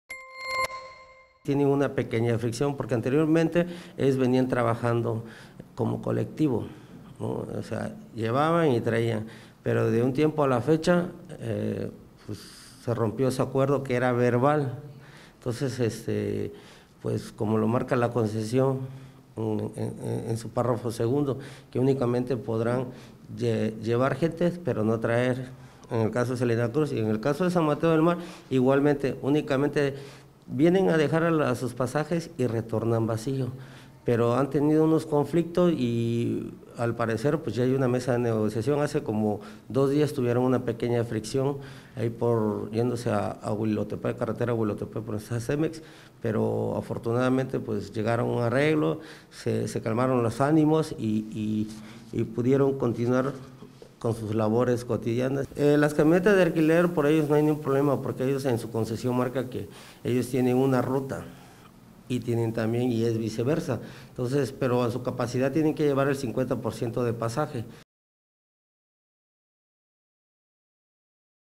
BITE CONFLICTO TAXIS
Luego del conflicto que se suscitó la primera semana del mes de marzo entre concesionarios de san mateo del mar y taxistas de salina cruz por motivos de pasajes ya que no contaban con la misma eficiencia por el hecho de tener dos rutas a lo cual el acuerdo verbal que mantenían se rompió,
Expresó el comandante de la policia vial adscrito a Salina Cruz  Rolando López que el desacuerdo que mantienen estos concesionarios se llevó a una negociación para hacerlo de forma conveniente para ambas partes de la cual obtuvieron una respuesta favorable